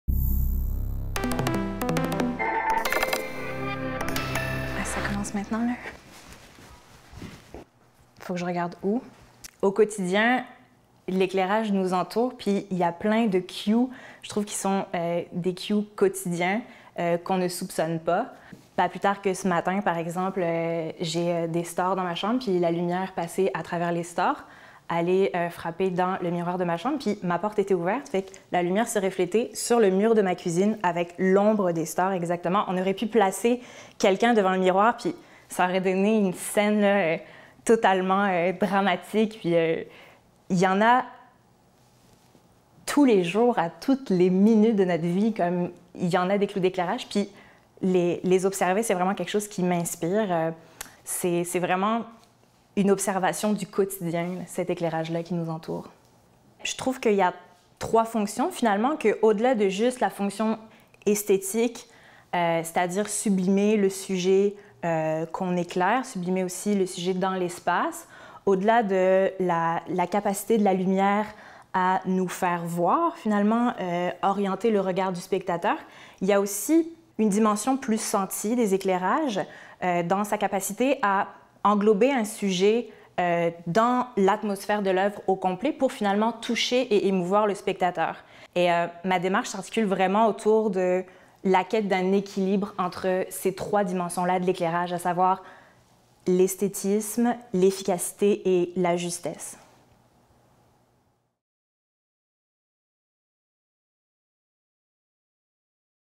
Julie Basse :: Conceptrice
Le JEU des 40

Née en 1989
Formée à l'École nationale de théâtre du Canada (2013)
Réalisations marquantes : PigeonsAffamés d’Anne-Marie White (Théâtre du Trillium, 2015), Un animal (mort) de Félix-Antoine Boutin (Création Dans la chambre, 2016) et Siri de Laurence Dauphinais et Maxime Carbonneau (La Messe Basse, 2016).

Fidèle collaboratrice de certains des plus avant-gardistes metteurs en scène de théâtre, cette conceptrice d'éclairages s'aventure également avec talent du côté de la danse, de l'opéra et de la musique populaire. Inspirée par tout ce qui l'entoure, au théâtre, au musée ou dans la rue, toujours en quête de justesse, elle emploie la lumière sur scène afin d'orienter le regard, d'instaurer des atmosphères enveloppantes, de susciter l'angoisse ou le réconfort, mais aussi de sublimer l'espace, de donner à l'instant une fulgurance, un caractère unique, inoubliable.